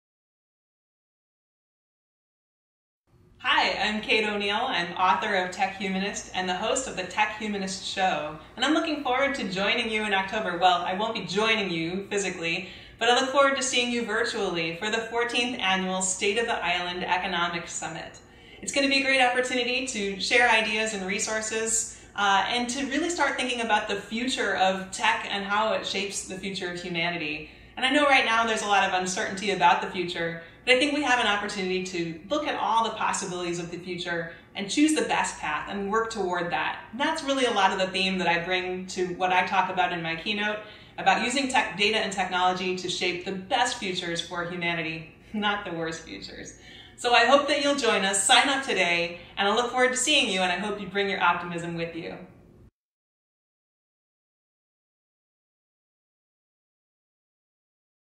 Tech Humanist Kate O'Neill Keynotes Upcoming Economic Summit
VIEA is thrilled to announce Kate O'Neill as a keynote speaker at our first-ever virtual Economic Summit, October 27-29, 2020.